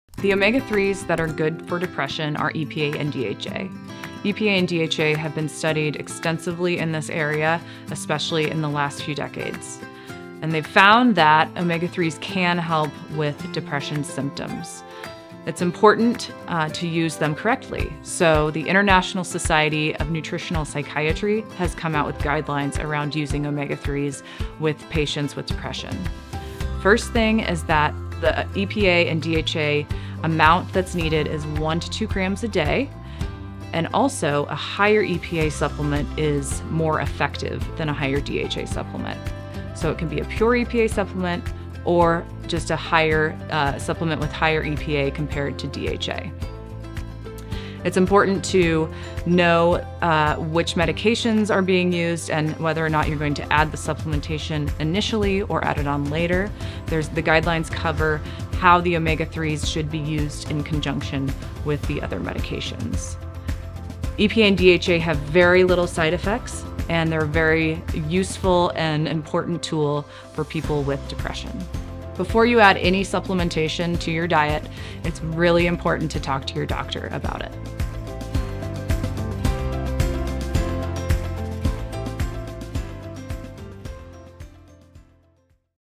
Which Omega 3 is good for depression?
The omega-3s that are good for depression are EPA and DHA. EPA and DHA have been studied extensively in this area, especially in the last few decades, and they've found that omega-3s can help with depression symptoms. It's important to use them correctly. So the International Society of Nutritional Psychiatry has come out with guidelines around using omega-3s with patients with depression. The first thing is that the EPA and DHA amount that's needed is one to two grams a day and also a higher EPA supplement is more effective than a higher DHA supplement. So it can be a pure EPA supplement or just a higher supplement with higher EPA compared to DHA. It's important to know which medications are being used and whether or not you're going to add the supplementation initially or add it on later. The guidelines cover how the omega-3s should be used in conjunction with the other medications. EPA and DHA have very little side effects and they're very useful and important tool for people with depression. Before you add any supplementation to your diet, it's really important to talk to your doctor about it.

Looking to make sure your diet is delivering the right amount of omega-3s?